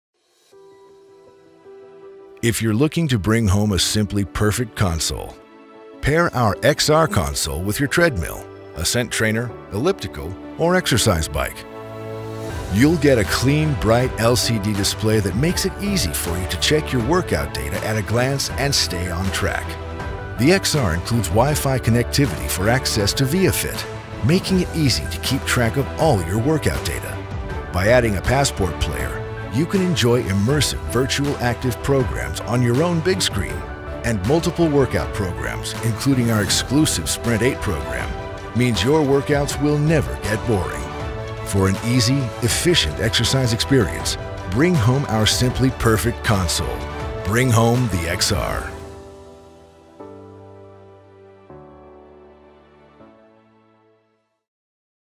Matrix XR Console featuring ViaFit
The Matrix XR console features a clean, bright display that makes it easy for you to check your workout data at a glance, keeping you on track to reach and surpass your goals.